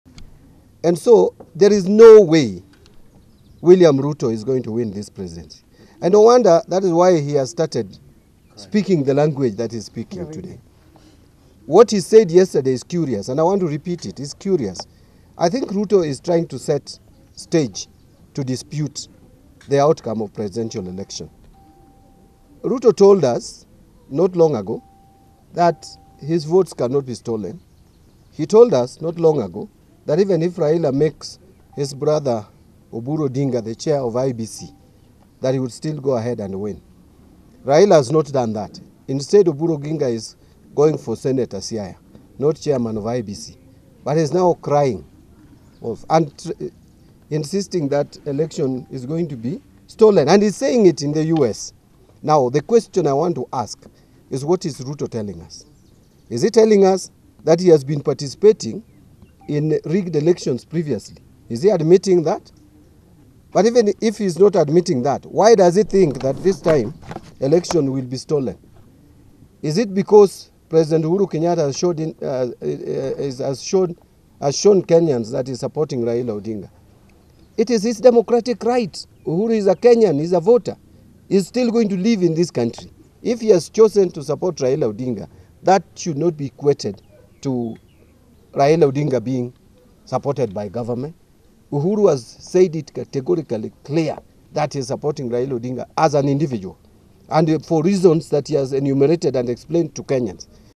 ODM Blasts Deputy President William Ruto Over Rigging Claims
Odm Party Has Blasted Deputy President William Ruto's Latest Remarks Claiming Of A Plot By The Executive To Rig The 2022 General Elections.Led By The Party Chairman John Mbadi,Odm Says Ruto Has Sensed Defeat Thus Resorted To Blame Games,Terming Them As Kicks Of A Dying Horse.The Dp Who Is Currently On Ten Day Us Tour,Expressed Discomfort Of Government Working With Opposition Which He Is Expected To Face Off On The 9th Of August General Elections.Ruto Wants Foreigners Intervene By Averting Any Attemps To Interfere With The Elections